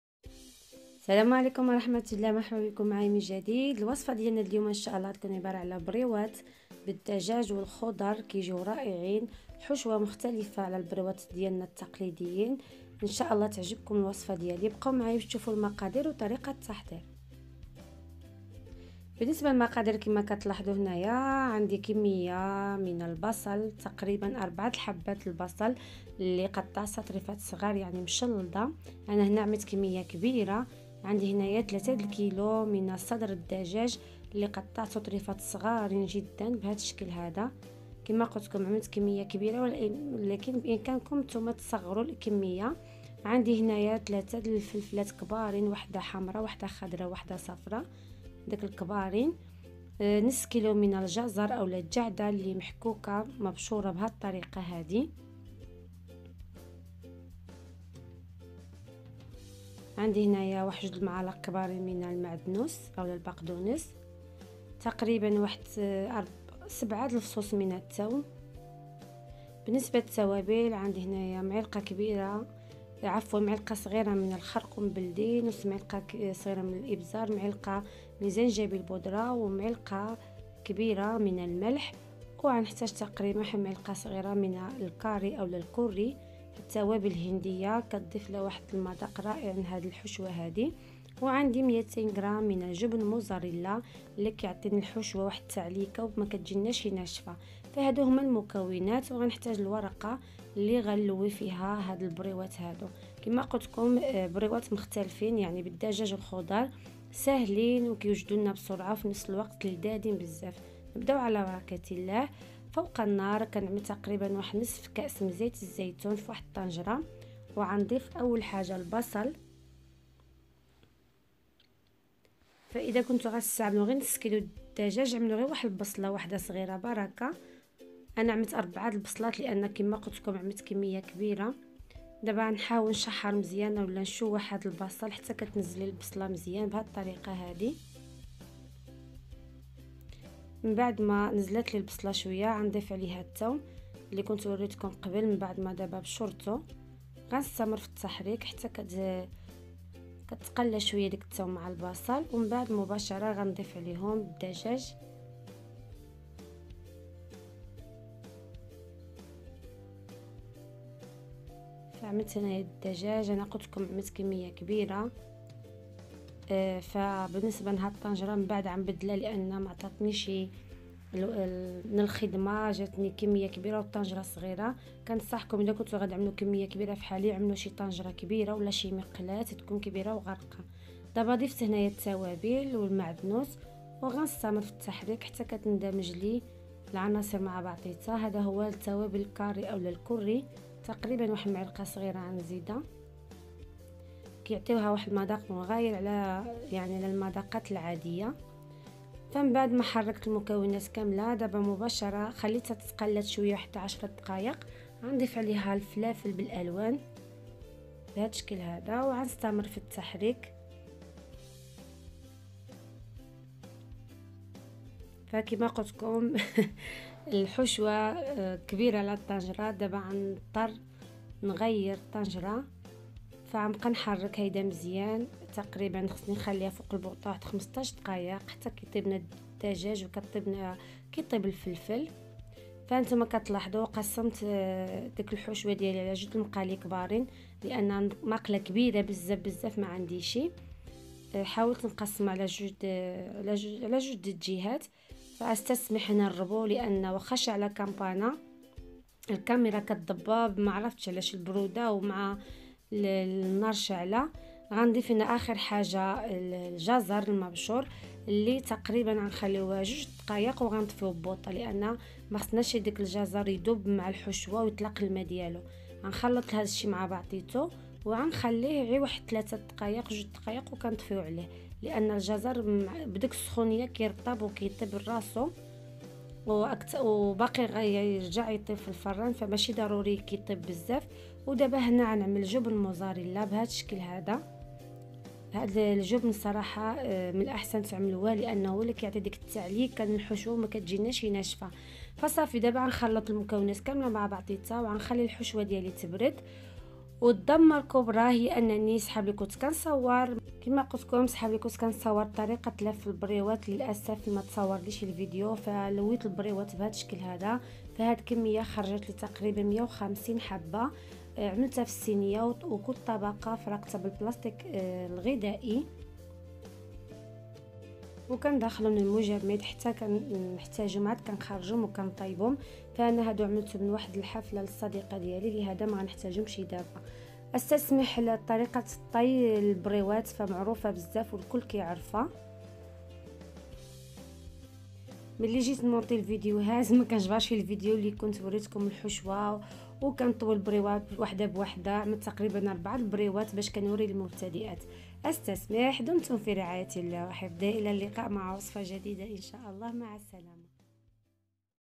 بريوات بالدجاج والخضر لذاذين واسهل ما يكون للمبتدءات
مقادير
صدر الدجاج الكمية المرغوب فيها
بصل
فلفل بالالوان
جزر بشور
ثوم
ملح فلفل اسود.باريكا.خرقوم.كاري.
جبن مزريلا
ورقة البسطيلة

مقادير :
 رابط طريقة عمل الدجاج والبصل للبسطيلة👇👇

وبالعافية